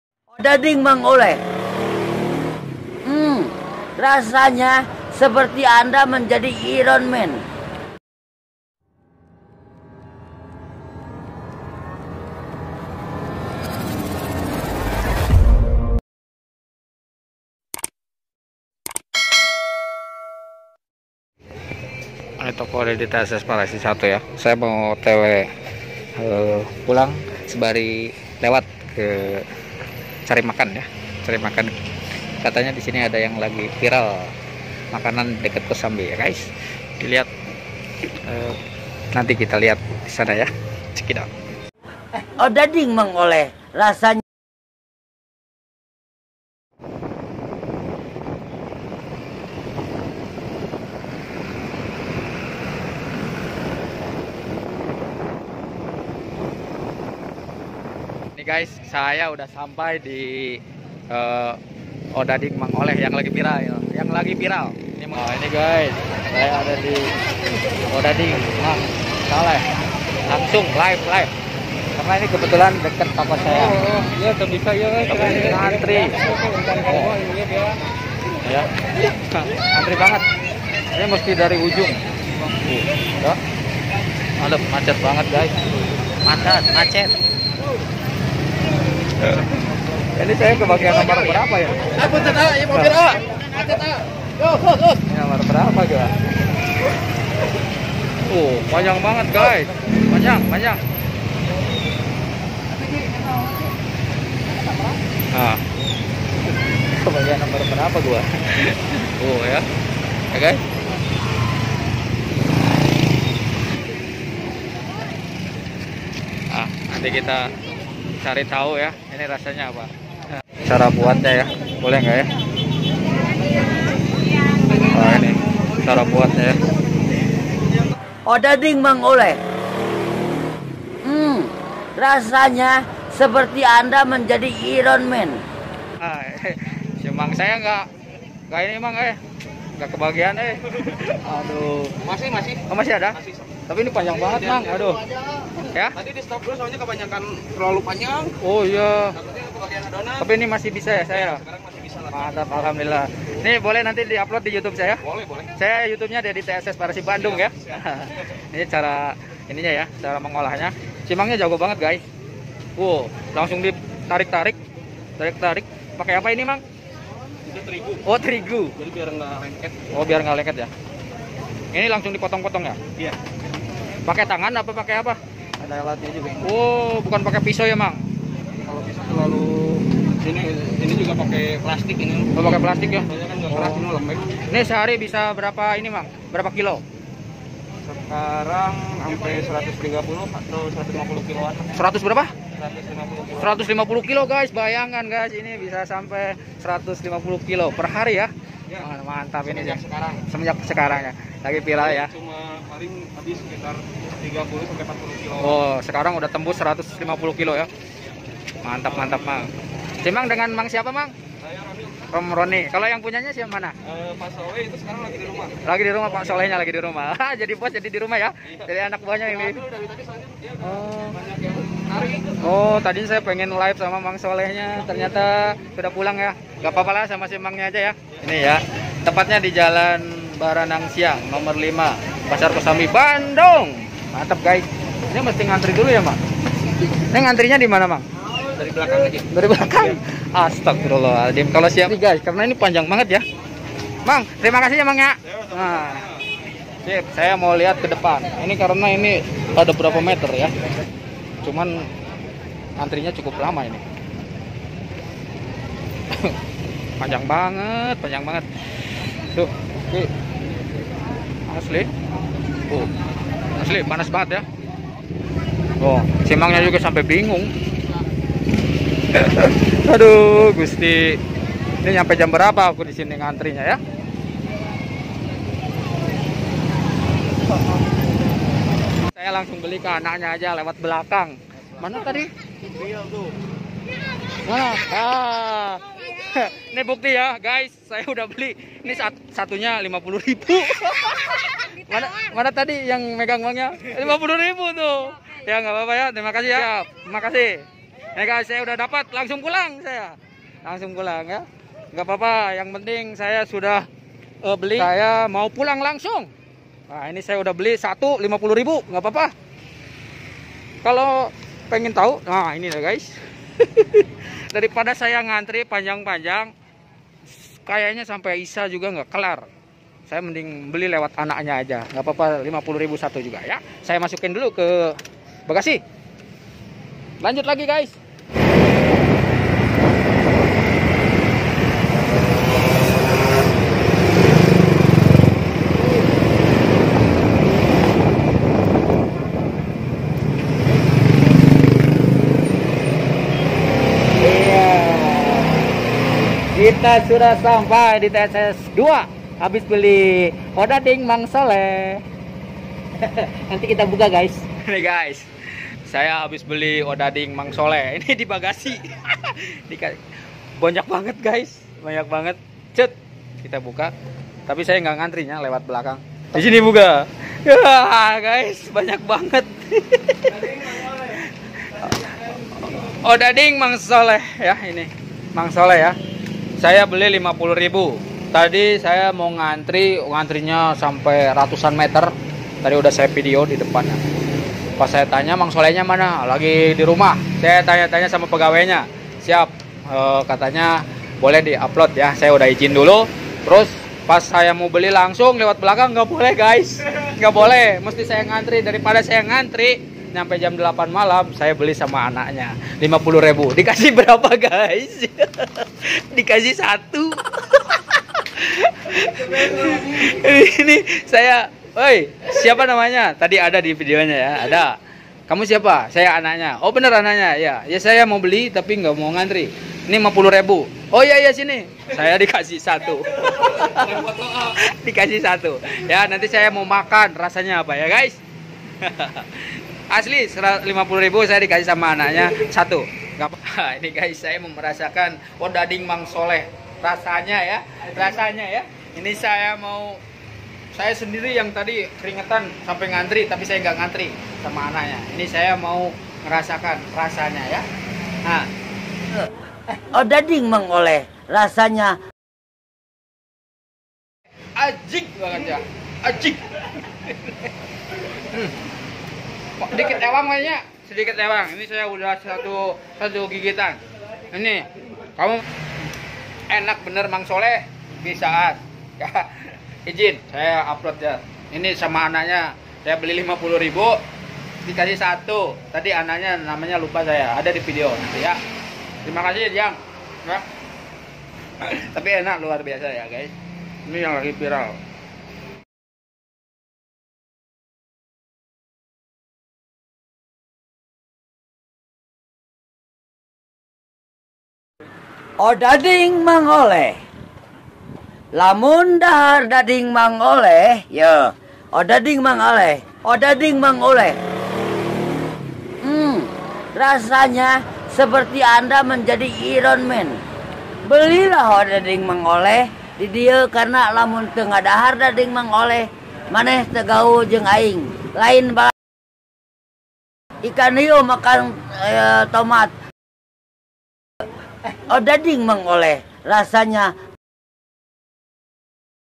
odading mang oleh viral | ngantri sampai 2 jam
odading yang lagi viral letaknya di jalan baranang siang kosambi bandung,dekat dengan toko dedy variasi 1 bandung , sebelum pulang  ke toko dedy variasi 2 mampir dulu ke odading mang sholeh, sayang antriannya panjang banget harus ngantri sekitat 2 jam,daripada ngantri sampai malam,mending beli sama anaknya 😊.

channel ini juga yang viralin  odading mang sholeh